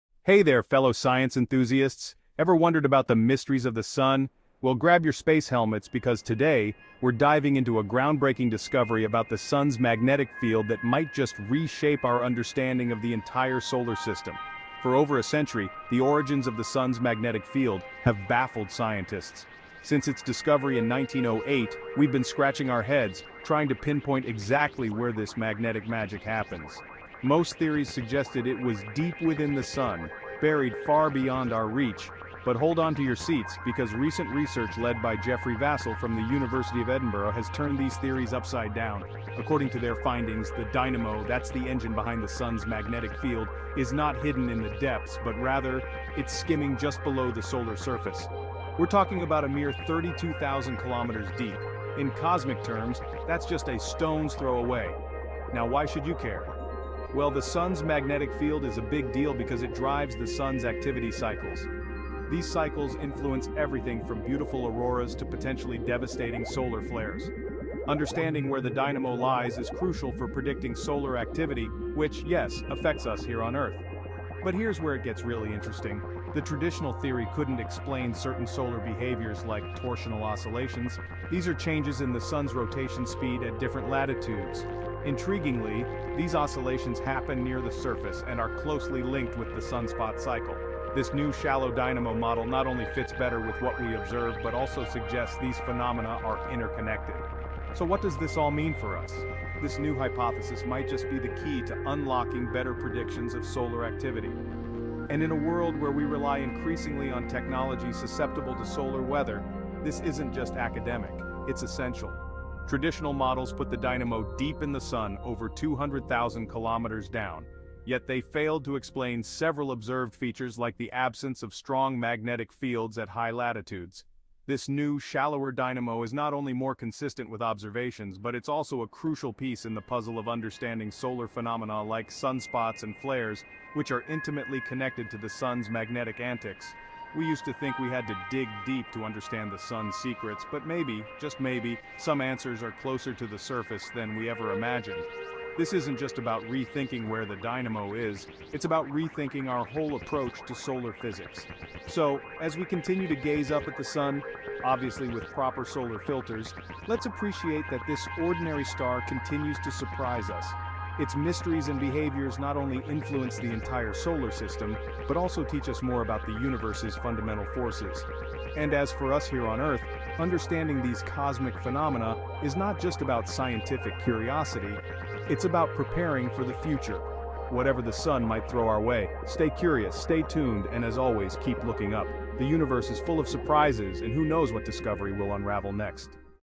SUN’S MAGNETIC FIELD IS NOT IN THE DEPTHS, BUT IT'S JUST BELOW THE SOLAR SURFACE
Source: This research published in Nature.
A century-long mystery in the world of science might be one step closer to being solved. It's all thanks to new research led by mathematician Geoffrey Vasil of the University of Edinburgh. We've been puzzling over the solar dynamo - the region in the Sun where the magnetic field is generated - since it was first discovered in 1908. And now, Vasil's team has discovered something fascinating.
Contrary to what we previously thought, the solar dynamo isn't hidden deep within the Sun. Instead, it sits surprisingly close to the surface, just 32,000 kilometers (20,000 miles) deep. This revelation is crucial because the Sun's magnetic field plays a vital role in its activity cycles. Understanding where the dynamo resides is key to unraveling the mysteries behind the solar cycles.
The Sun's magnetic field is an intricate and ever-changing system that has left researchers perplexed for years. By studying Earth's magnetic field, created by a dynamo in its outer core, we've gained some understanding of how magnetic fields are generated. However, the Sun's internal workings are far more complex and challenging to study.
Solar activity, like sunspots, flares, and coronal mass ejections, has a direct impact on Earth. Scientists have observed that this activity ebbs and flows in cycles lasting roughly 11 years. During solar maximum, the Sun is at its most active state, with intense sunspot and flare activity, and even a reversal of its magnetic poles.
In recent years, researchers have started to question the traditional theories about the Sun's magnetic field. By conducting detailed simulations based on real data, Vasil and his team proposed a new idea - the dynamo responsible for the Sun's magnetic field might be closer to the solar surface, in what is known as the near-surface shear layer.
This new hypothesis not only aligns better with observations but also offers a fresh perspective on the Sun's behavior. It challenges the long-held belief that the dynamo resides deep within the Sun and provides a new path for understanding the complexities of our closest star. By thinking outside the box, scientists are paving the way for groundbreaking discoveries in solar physics.